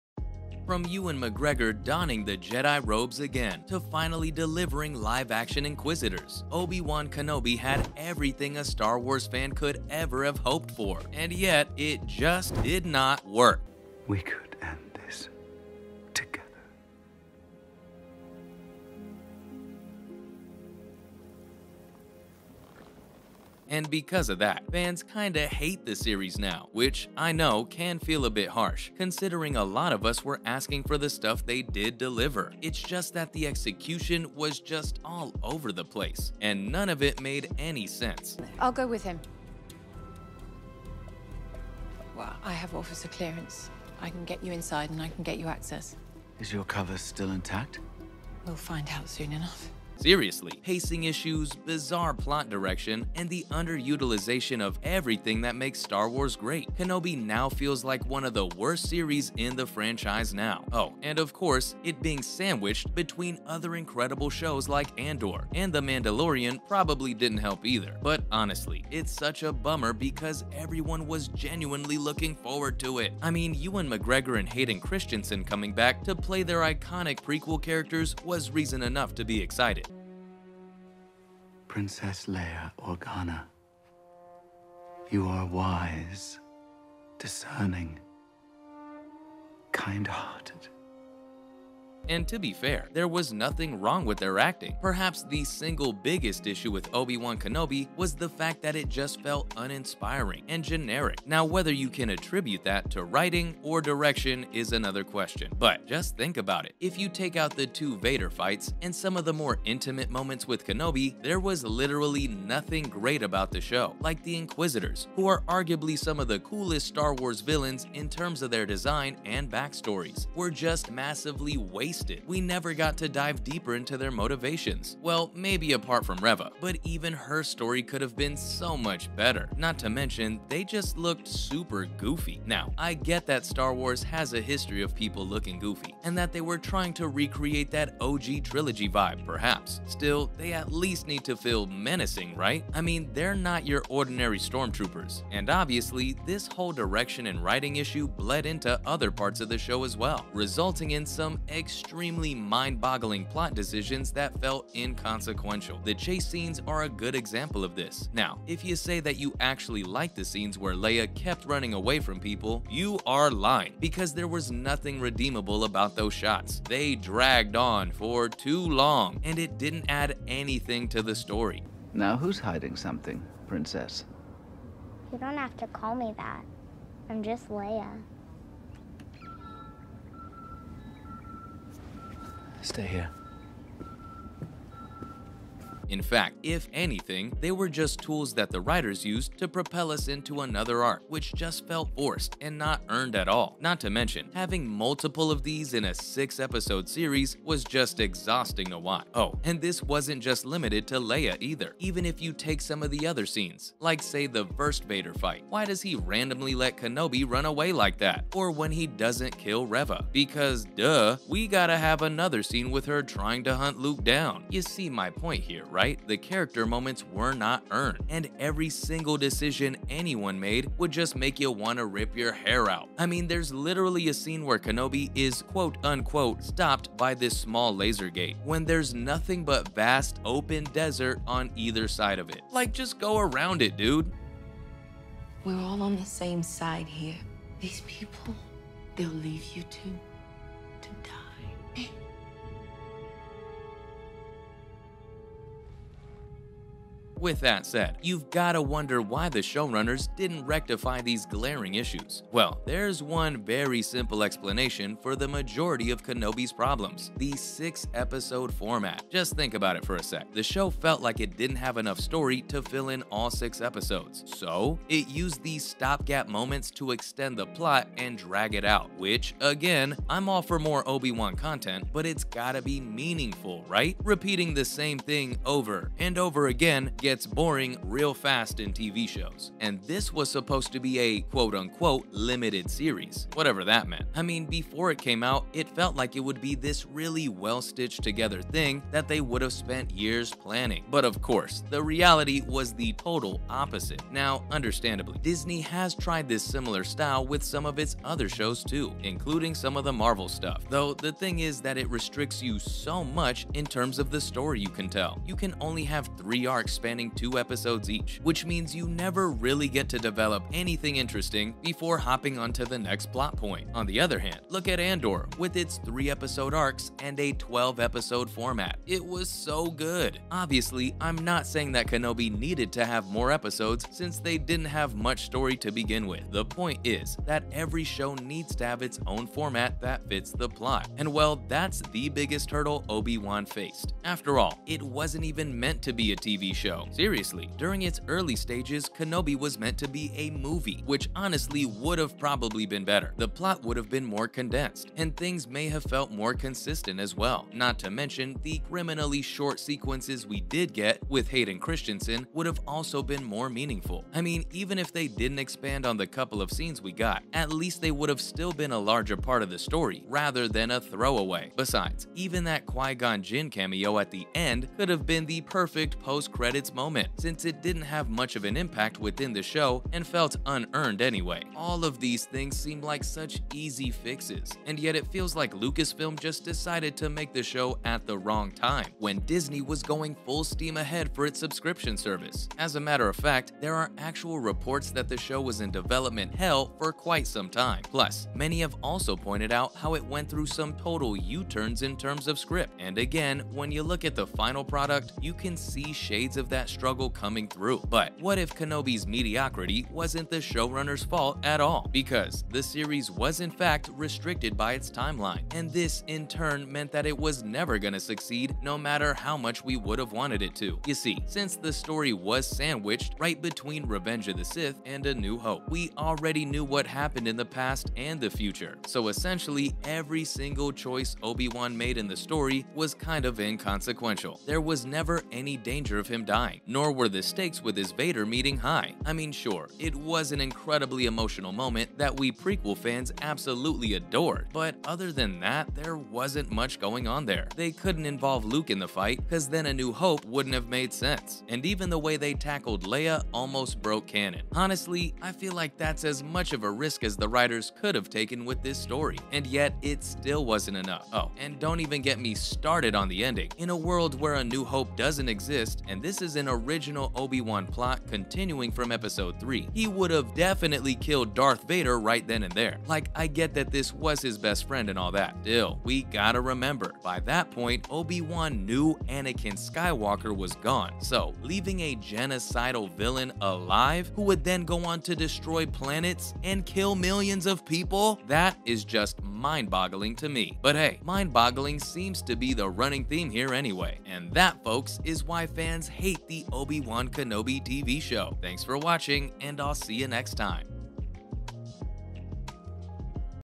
Fans HATE The Obi Wan Kenobi Series.. Here's Why!
Welcome back to Film Pop, today on the channel we're going to be talking about Fans HATE The Obi Wan Kenobi Series.. Here's Why! if you're excited to learn more about this then make sure you watch the whole way through because you don't want to miss the details we have for you!